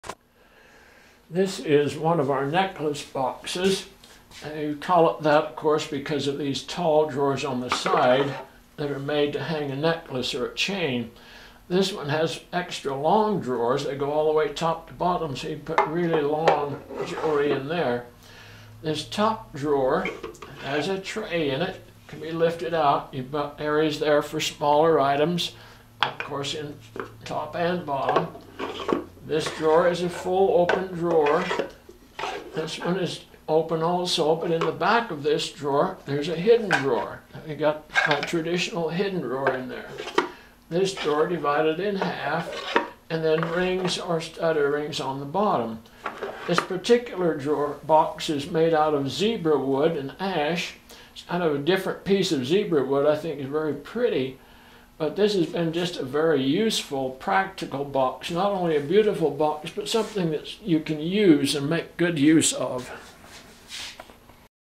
Old Time Radio Shaped 7 Drawer Band Saw Box
2) - ETSY - NECKLACE JEWELRY BOXES

This Antique Radio shaped band saw box has two tall drawers for hanging necklaces or chains. The top drawer has a lift out tray.  There is also a hidden drawer and the bottom drawer is set up to display rings.  This box, like all my boxes, is solid wood and hand crafted to last a lifetime.  They are all unique and one of a kind, even two made from the same pattern.